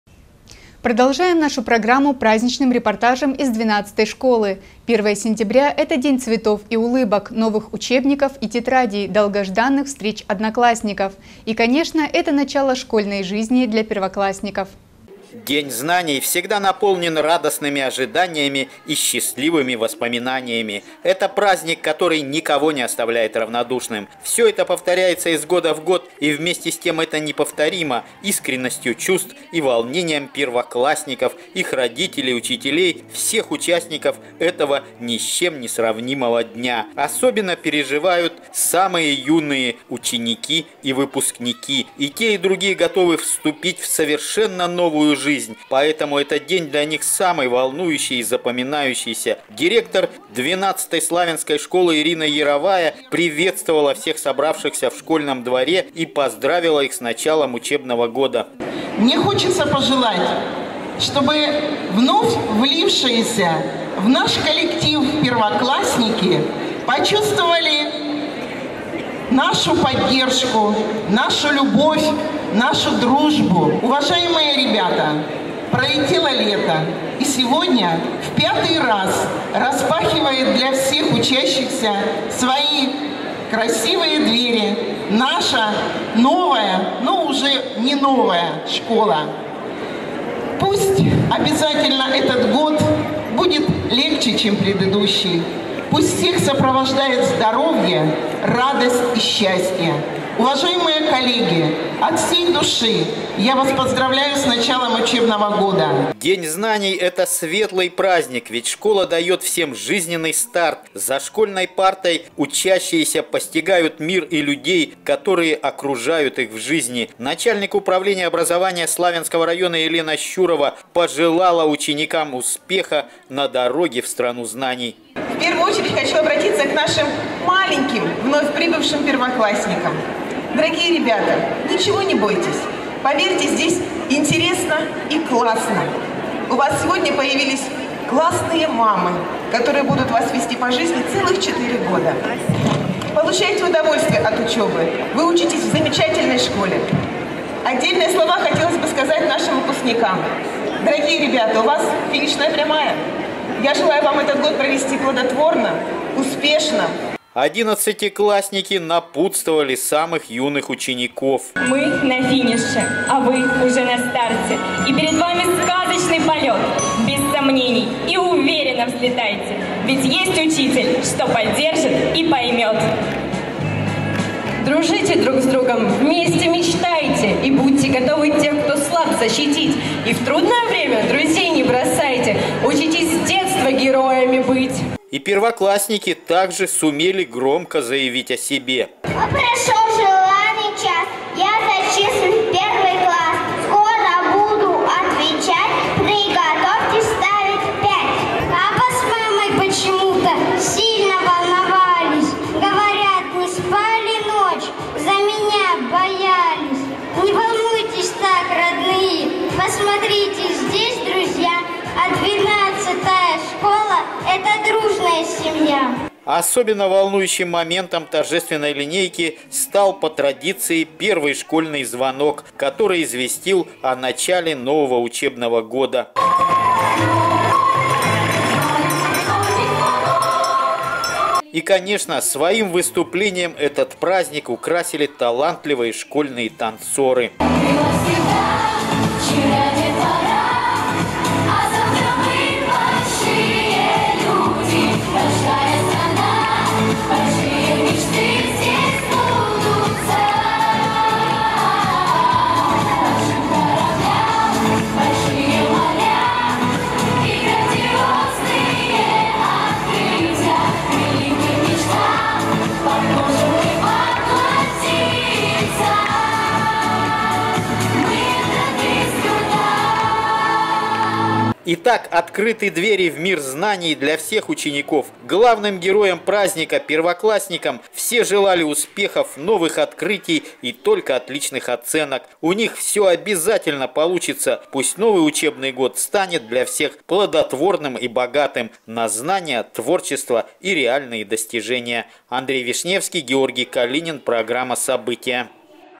На пороге новых ярких открытий
Первое сентября – это день цветов и улыбок, новых учебников и тетрадей, долгожданных встреч одноклассников. И, конечно, это начало школьной жизни для первоклассников